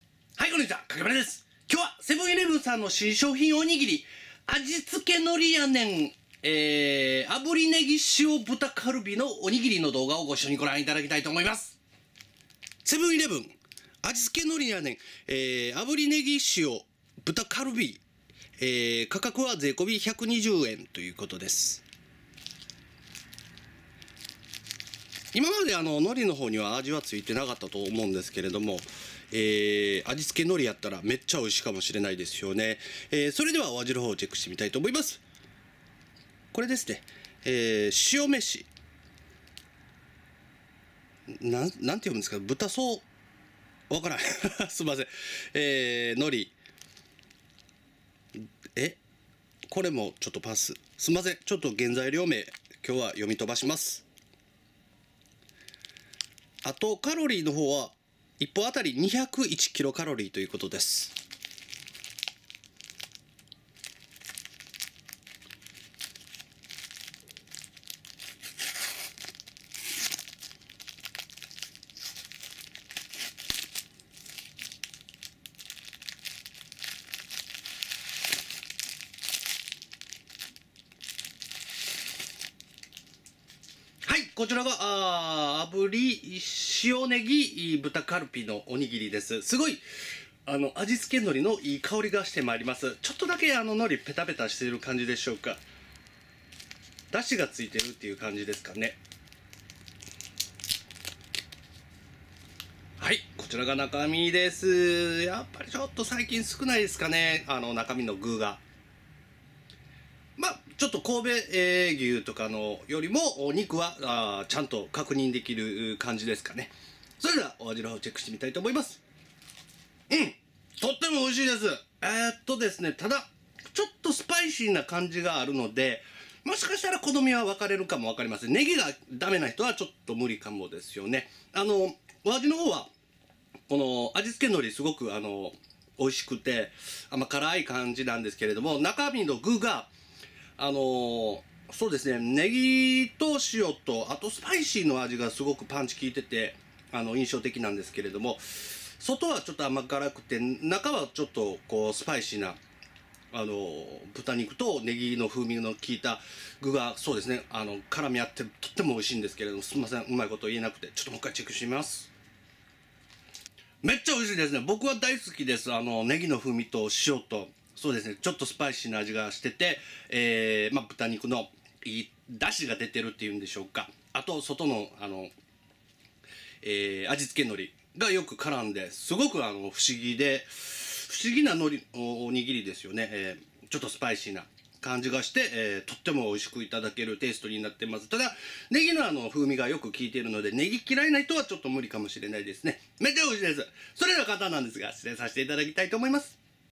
セブンイレブンのおにぎり 炙りねぎ塩豚カルビを食べてみた
セブンイレブンのおにぎり 炙りねぎ塩豚カルビを購入し食べた感想を解説した動画です。